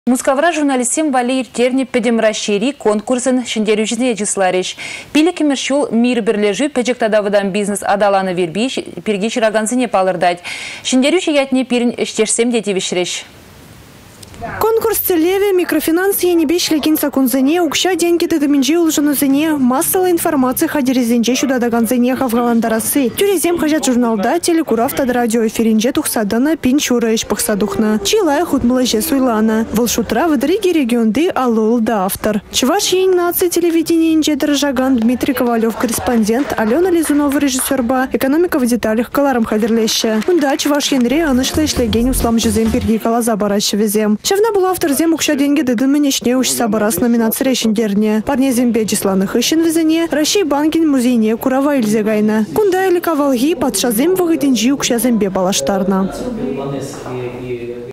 Чăваш Ен наци телевиденийĕн хастар ĕçченĕсем
Мускавра журналистсем валли ирттернĕ пĕтĕм Раççейри конкурсăн çĕнтерӳçисене чысларĕç. 5-мĕш çул «Мир» пĕрлешӳ пĕчĕк тата вăтам бизнес аталанăвĕ пирки çыракансене палăртать. Çĕнтерӳçĕ ятне пирĕн ĕçтешсем те тивĕçрĕç.